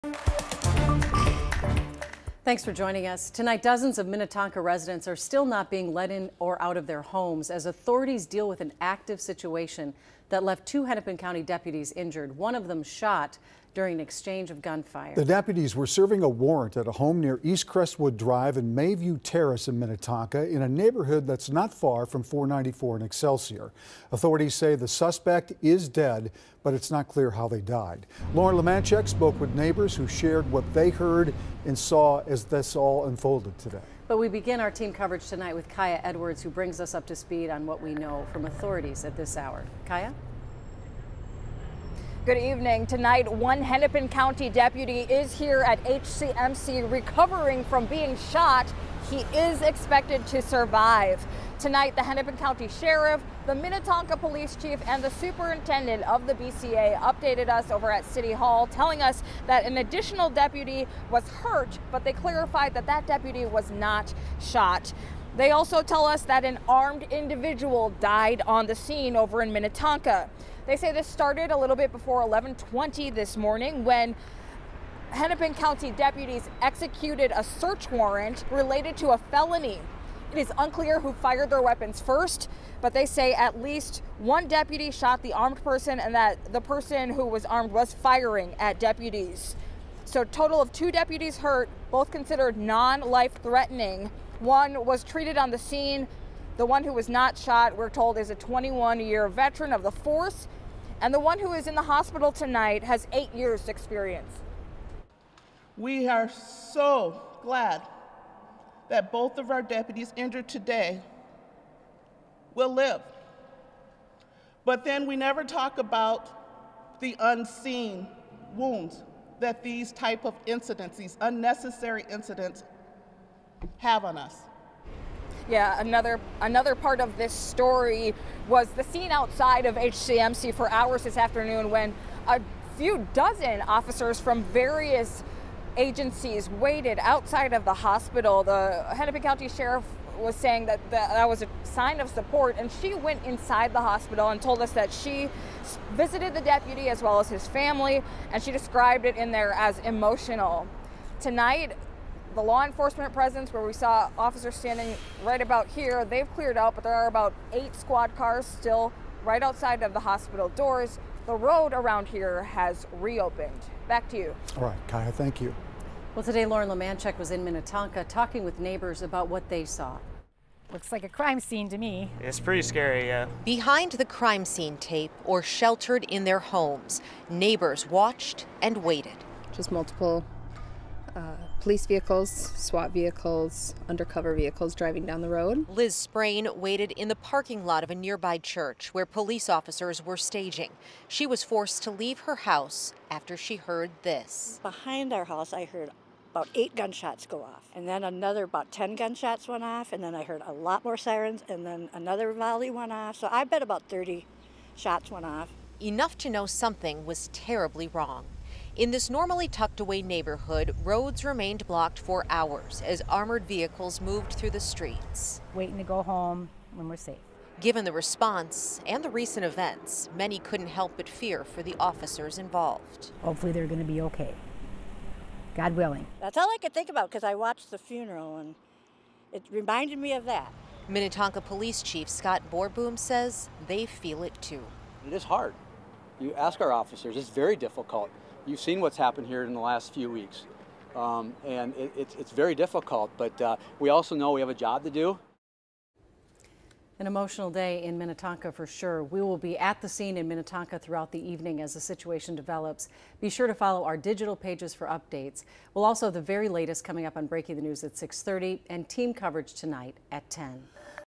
Police: Hennepin County deputy shot, another hurt serving warrant in Minnetonka
A Hennepin County deputy was shot and another deputy was hurt while serving a warrant in Minnetonka on Wednesday.